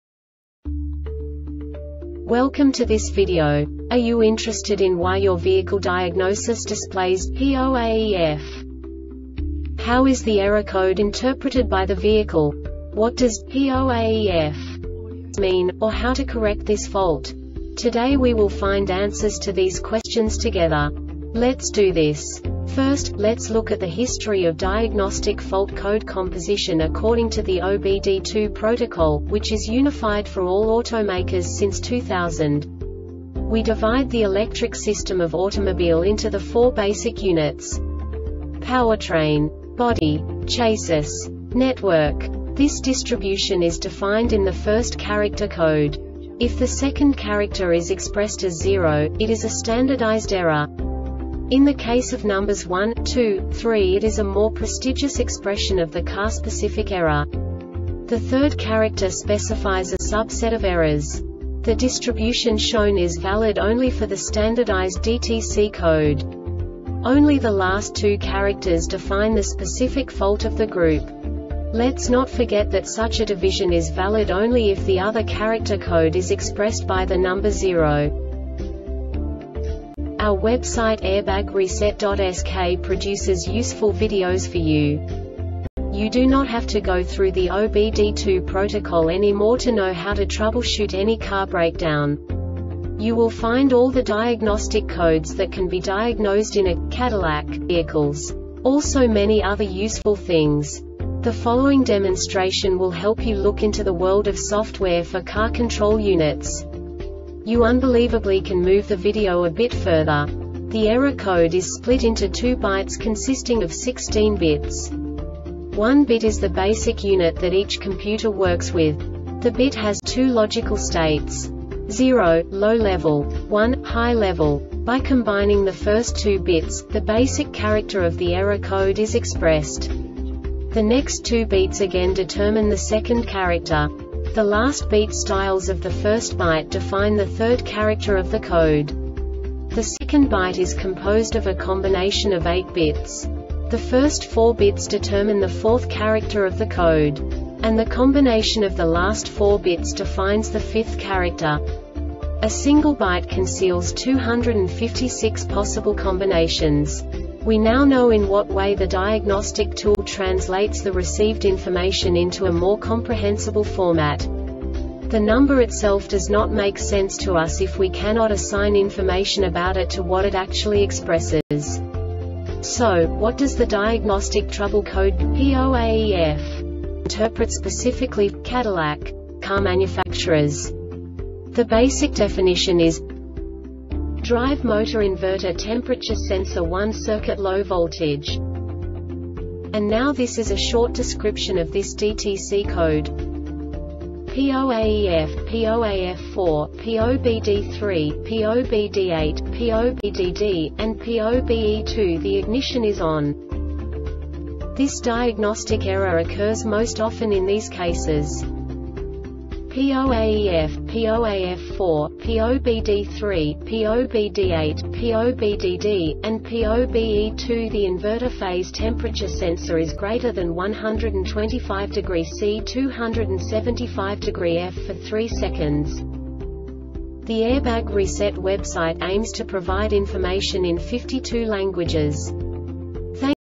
DTC cadillac P0AEF Short Explanation
Contents:
0:21 Basic DTC analysis according to OBD2 protocol standard.
1:48 Insight into programming
2:58 A diagnostically understandable description of the Diagnostic Trouble Code
3:05 Basic error definition

Make:
Cadillac
Code:
P0AEF
Definition:
Drive Motor Inverter Temperature Sensor 1 Circuit Low Voltage
Description:
P0AEF, P0AF4, P0BD3, P0BD8, P0BDD, and P0BE2 The ignition is ON.
Cause:
P0AEF, P0AF4, P0BD3, P0BD8, P0BDD, and P0BE2
The inverter phase temperature sensor is greater than 125°C (275°F) for 3 seconds.